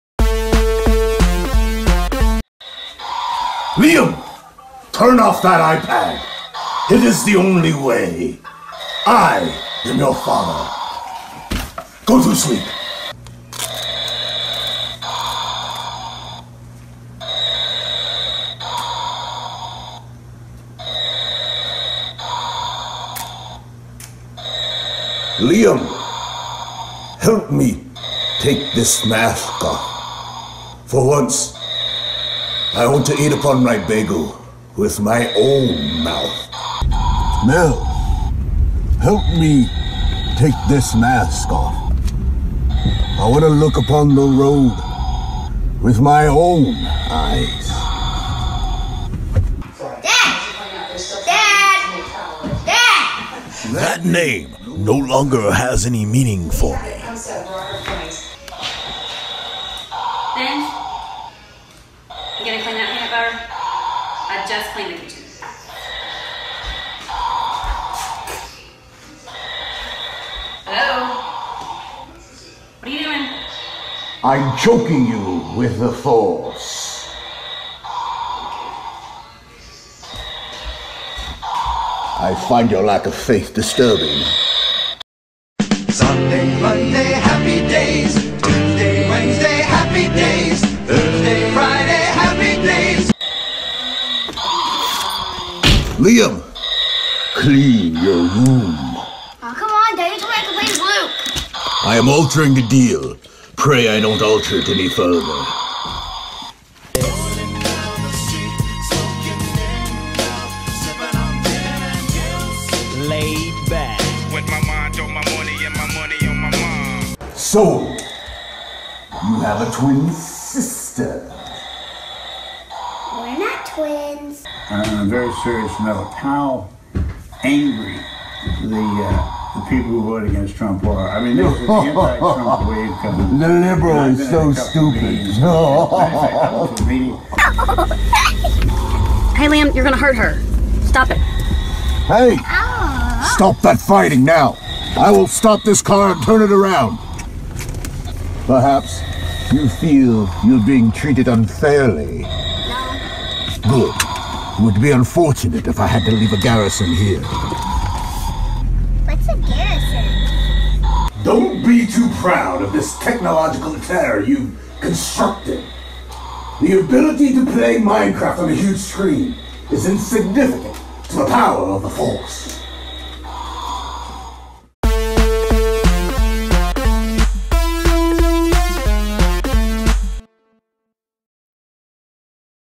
Darth Daddy (Star Wars, Darth Vader, Rogue One)
Dad is a super fan of any and all things Star Wars and harasses family with obsession with being Darth Vader ... aka ...Darth Daddy.

(Star Wars, Darth Vader, Darth Daddy, Rogue One, The Last Jedi)